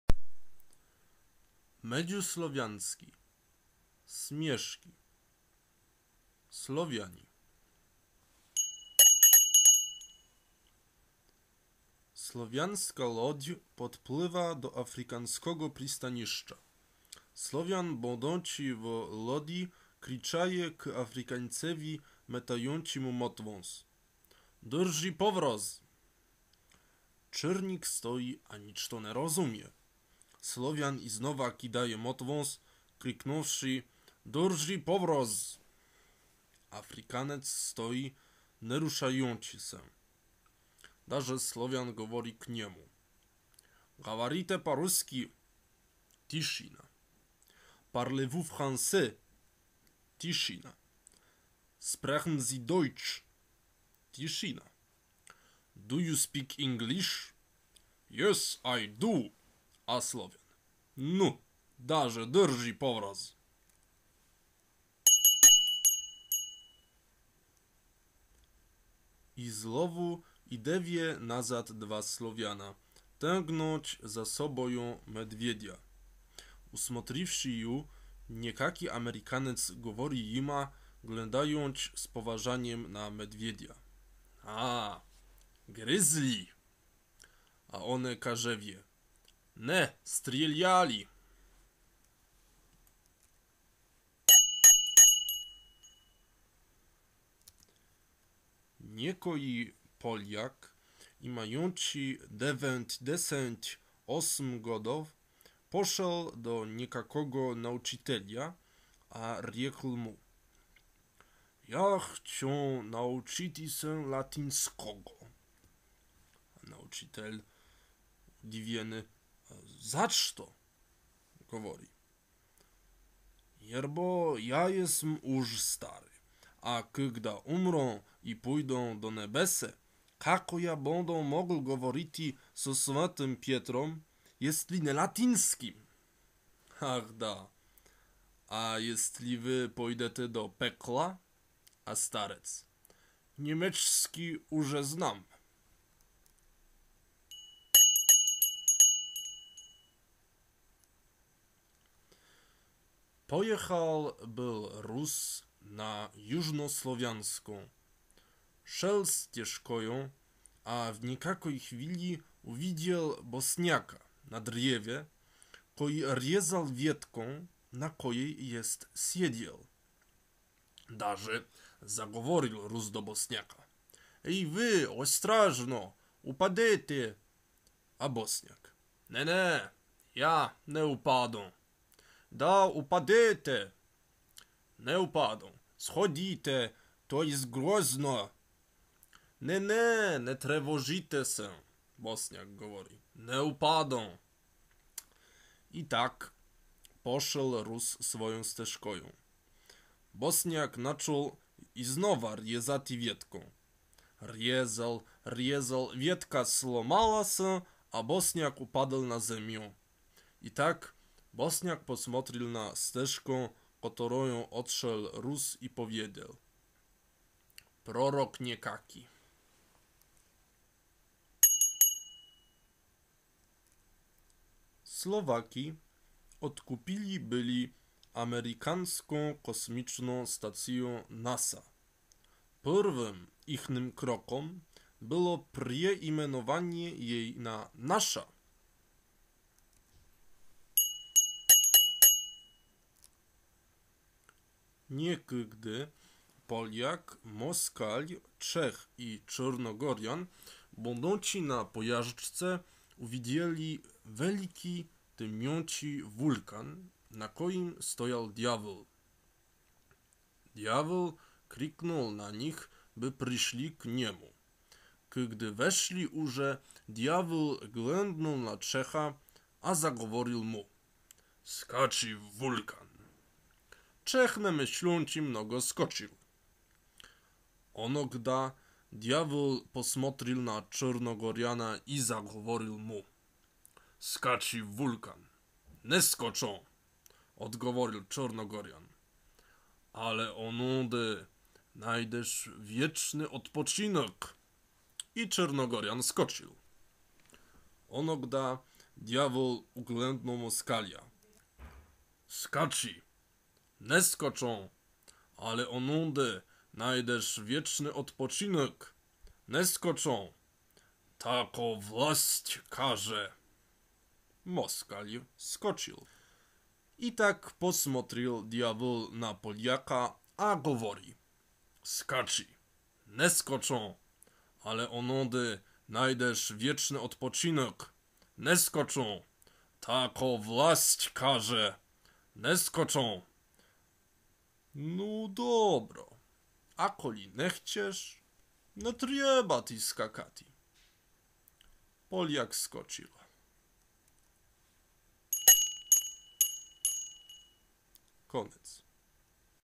(Old ISV Version)Medžuslovjanski: Směški - Slovjani/Smijesni vicevi/vtipy/смешные шутки/smesni/жарти
(Old ISV Version)

Zvězdka pri slovesi (*) znači, iže slovo ne jestvuje vò Međuslovjanskom Rěčniku.

*v sej směškě je upotrěbjana dvojina

PEKLO/PEKELO(?)

anglijski: hell ;
bělarusski:Пекла ; bulgarski:Ад ; česski:Peklo ;  hrvatski:Pakao ;
poljski:piekło ;praslovjanski:  ;rusinski: ;  russki:Ад  ;
 slovačski:Peklo ; slovenačski:Pekel ; sŕbski:Пакао ;
 ukrajinski:Пекло ;

*Južnoslovjanska - od slovese "Slovjanska" ne ide ob Jugoslavijų, ale ob zemje na kojih
sejčas domovijųt Južne Slovjani.

Međuslovjanski ne jest SLOVIO
INTERSLAVIC MEDŽUSLOVJANSKI SLOVIANSKI MEDŹUSLOVJANSKI Słowianie Славяне
Славяни Slované Słowjany Słowiónie Slaveni Słowjenjo Словени Славяне
Slovania Slovani Слов'яни